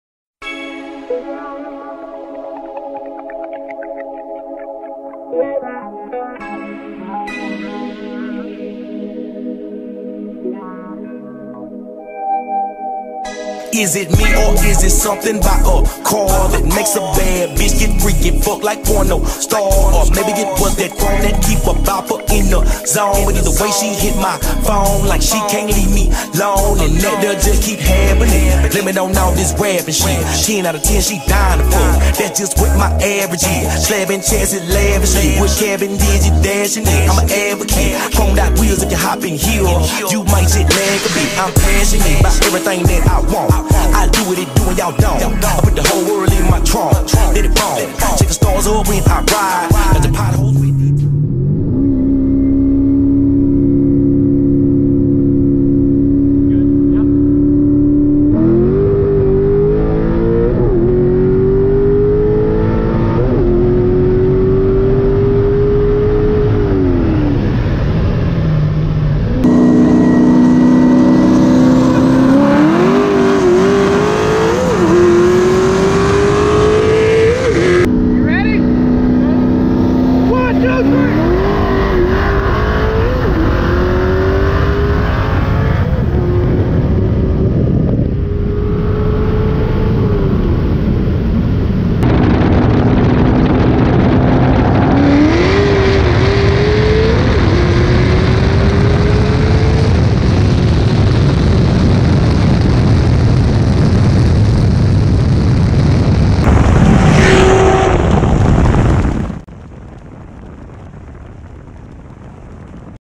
C6 Z06 vs 2019 TT 5.0!
Clifford vs a newly built twin turbo 5.0!
Next mods for the 5.0 will be converter and trans/rearend upgrades, then more boost!
Cant wait for the rematch 🤘🤙